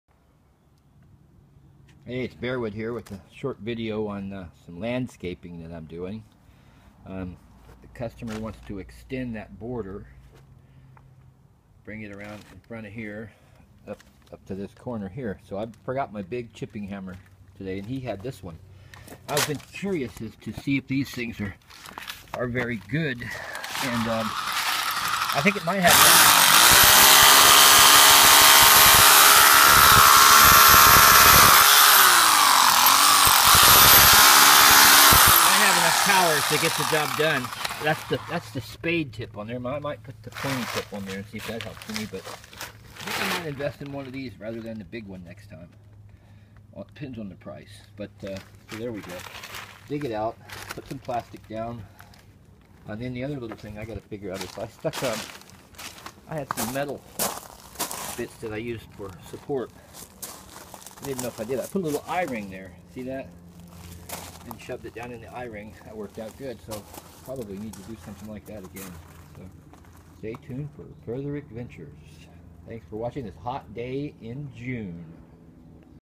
Landscaping TOOLS Small SDS chipping hammers England.
Bearwood Road Berkshire  Digging in the garden  dirt on a HOT day June 5 2016  LOL  for England   probably  75 F  actually  it so far was maybe  the nicest day we have had  been raining again